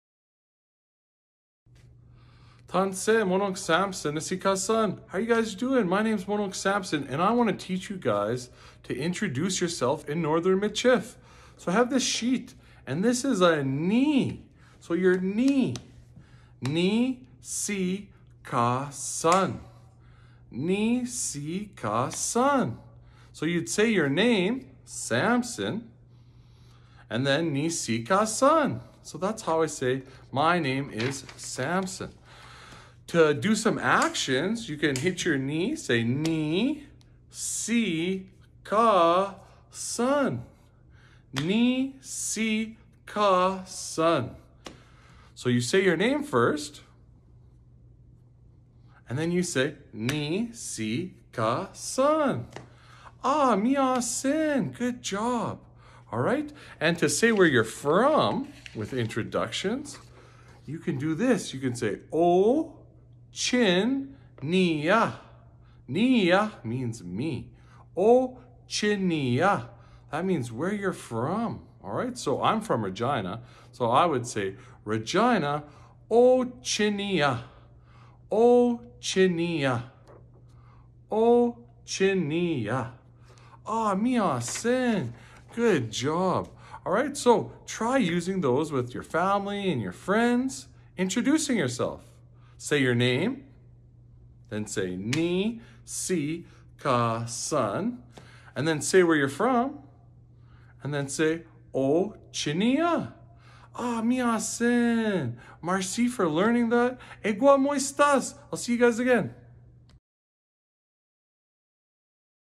Northern Michif Minute: Introductions
Learn Northern Michif in this fun series with Elder Louise Oelke and Samson LaMontagne! In this video, we're learning how to introduce ourselves.

Louis Oelke is from Sâķitawahk– Ile a La Crosse Saskatchewan. She speaks Northern Michif and is a Traditional Métis Knowledge Keeper within our Community of Saskatoon. She has 5 children, 14 grandchildren and 6 great grandkids. She's a mom, a kokum and a chapan!

Samson LaMontagne is from Oskana Kâ Asastîķi - Regina, Saskatchewan. He has been learning Northern and Southern Michif for the past 8 years from Michif Knowledge Keepers within our Community of Saskatoon. He has a beautiful daughter, an amazing wife and a supportive family. He is a father, a husband and a teacher!